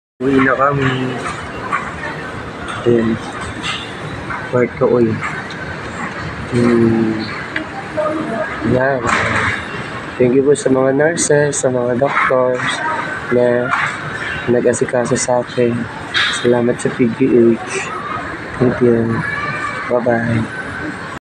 April.27,2024 | Discharge Na Po Ako Sa PGH - Ellowe Alviso
April.23,2024

This video 📷 caught on April.27,2024

Lubos Po Ako Nagpapasalamat Sa Mga Doctors ko Sa Psychiatrist, Otorhinolaryngology,Nurses,Staft,Malasakit at Mga Doctors ko na Nag Opera Po Sa Akin Lalo na Po Kay Doctor Mendoza at Sa Mga Kasamahan niyang Doctors din Po nag asikaso Po Sa akin Sa operating room Mga Anesthesialogy.Maraming Salamat din Po Kay Doctor.Lacsama Sa Pag Asikaso Po at Paglilinis Po Ng Aking Sugat Maraming Salamat Po God Blessed Po.